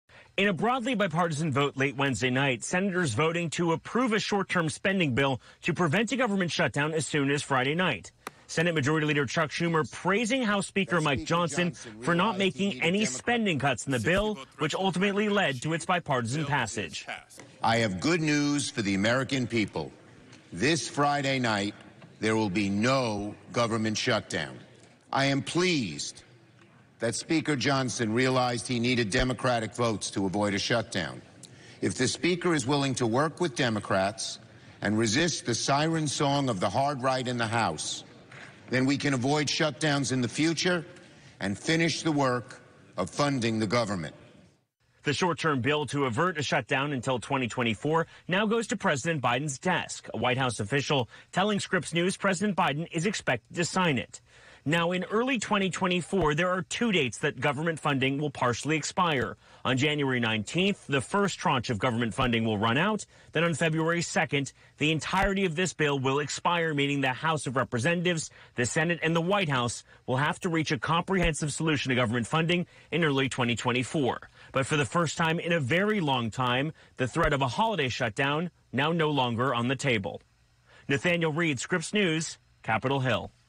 Senate avoids government shutdown by approving temporary spending bill
The Senate gave final approval to a temporary government funding package Wednesday night and sent it to President Joe Biden for his signature, ending the threat of a government shutdown until after the holidays.

With only days to spare, the Senate worked into the night to pass the bill before government funding expires Saturday. The spending package keeps government funding at current levels for roughly two more months while a long-term package is negotiated.